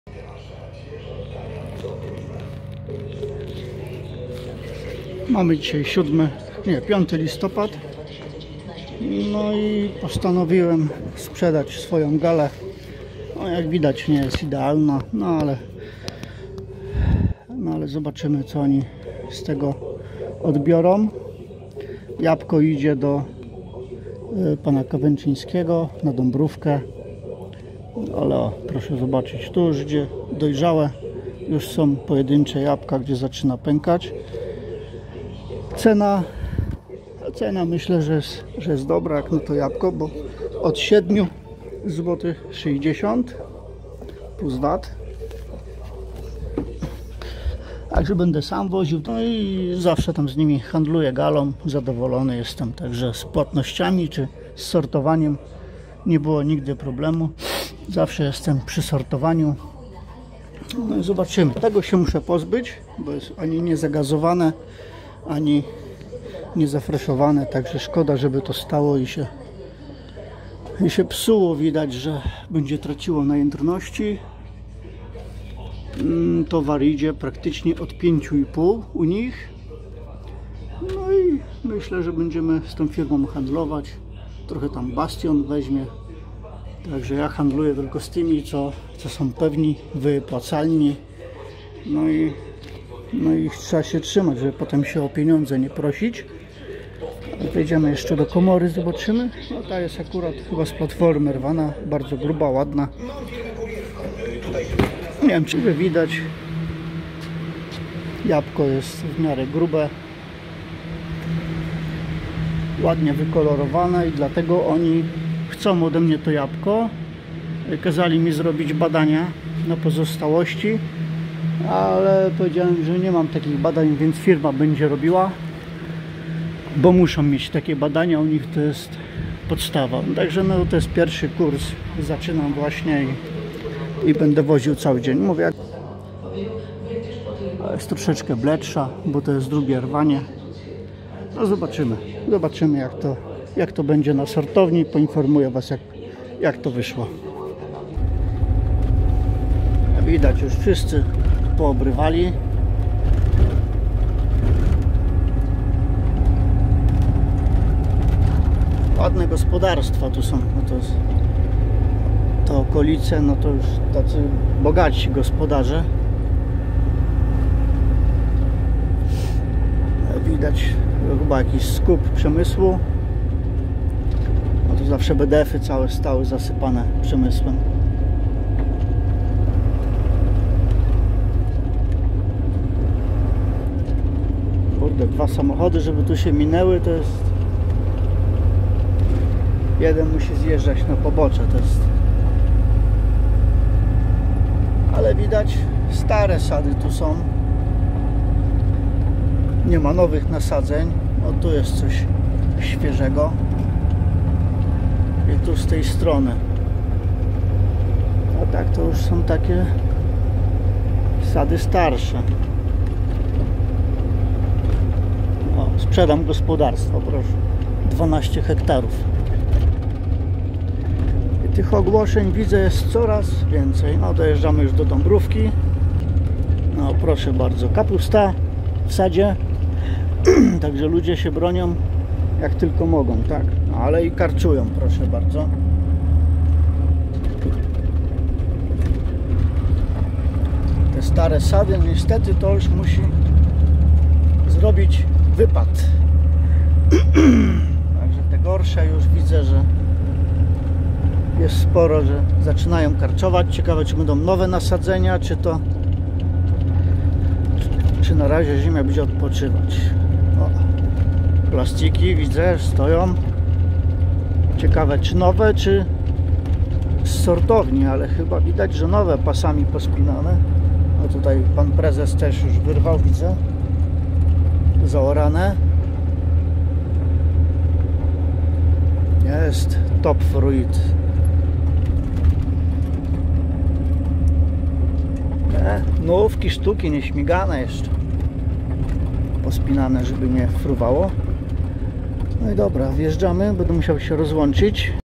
Handel pierwszy #68 Komunikat sadowniczy z 5.11.2019 Paweł Kazana
Opróżniam pierwszą komorę. Zobaczymy jak od sortowałem po tym gradzie.